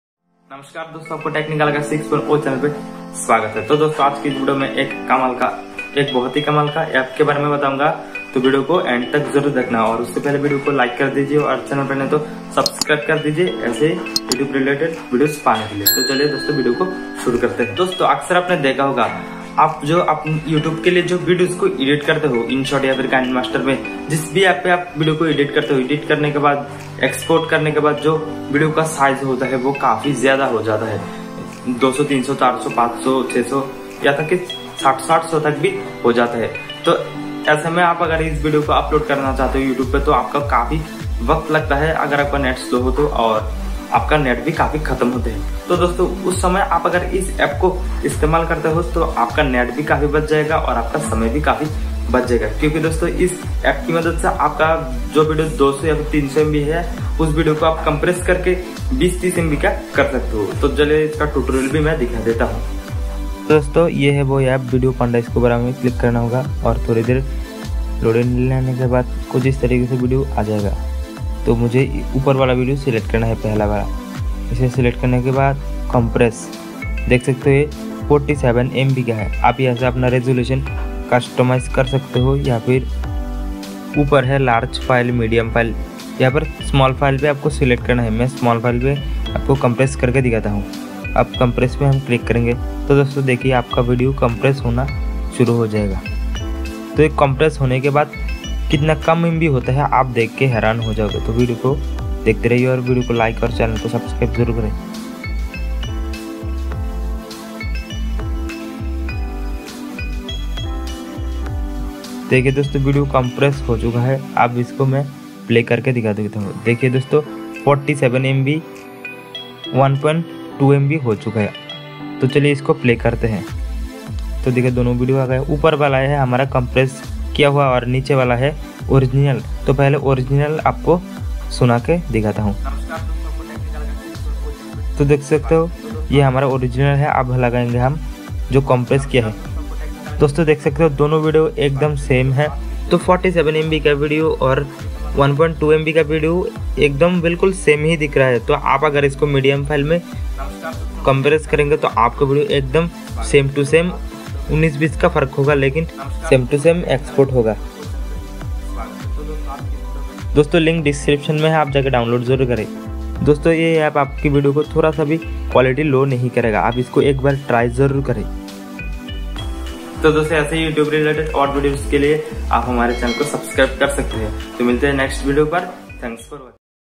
Video Compress Mobile App | best video compressor for Android | Technical Akash 6.o
about this video:
hello friends in this video I am talking about how can you compress your YouTube videos without less quality in video Panda application.